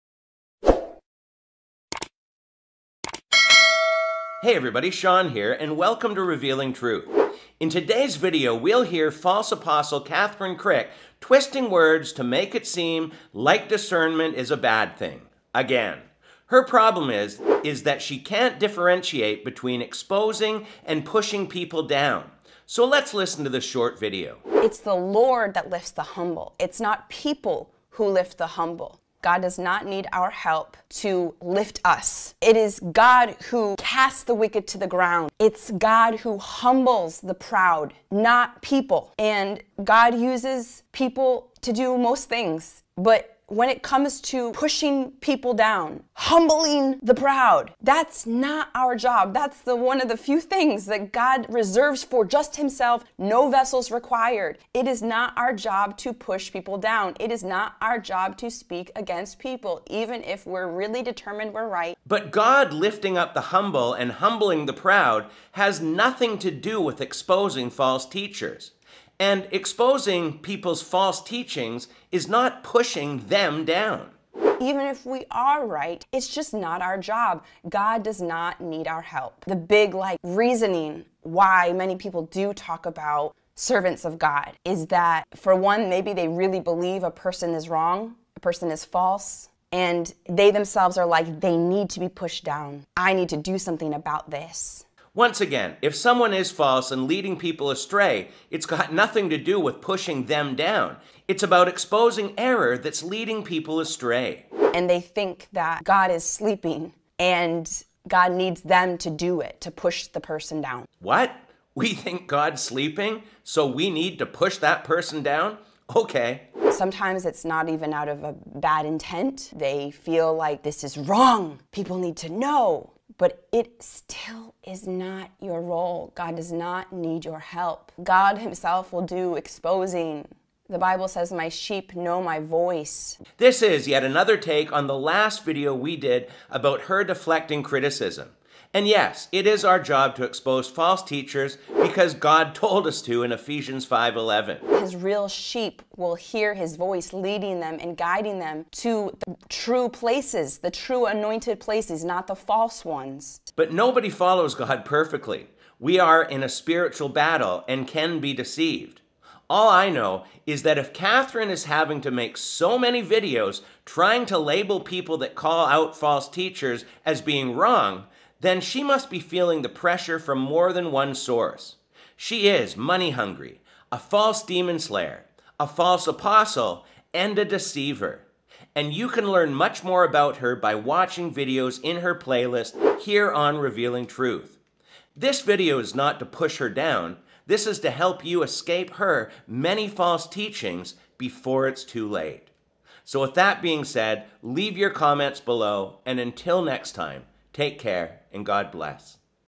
Kathryn Krick Seems VERY WORRIED
Only someone that is feeling the pressure of being false, would have to make so many videos labelling exposers as being unbiblical.